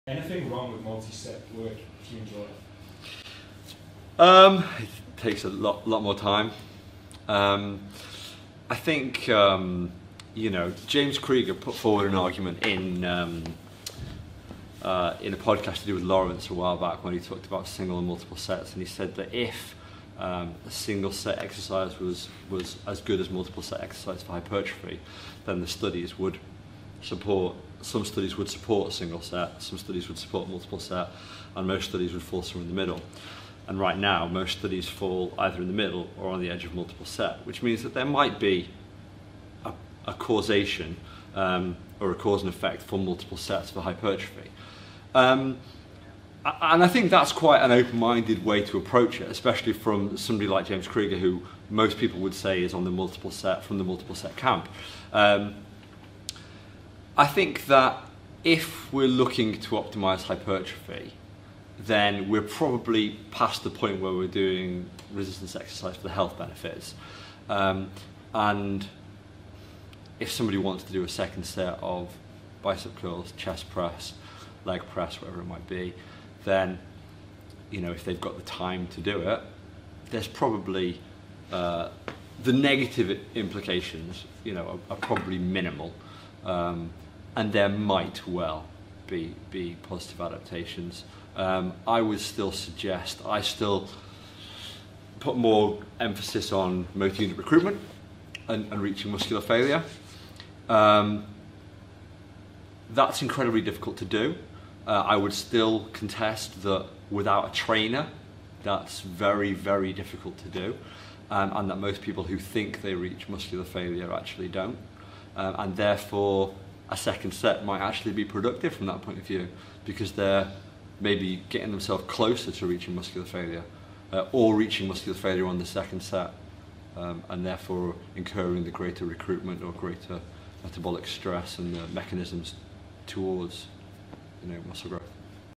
Séries ÚNICAS VS MÚLTIPLAS de um exercício
APOIE O CANAL DO SIMPLES E FLEXÍVEL NO YOUTUBE

James Fisher fala sobre o número ideal de séries por exercício em um treino. Ele recomenda realizar uma única série por exercício até a falha muscular momentânea. Indivíduos que treinam sem a supervisão de um personal trainer podem não conseguir atingir a FMM, caso em que uma segunda série pode aproximá-los e fornecer um estímulo maior.

James Fisher é Professor Sênior de Condicionamento Esportivo e Fitness na Southampton Solent University

A HITuni é especializada na oferta de cursos de certificação online em Treinamento de Alta Intensidade e Exercício de Resistência.

Curtam, comentem e compartilhem!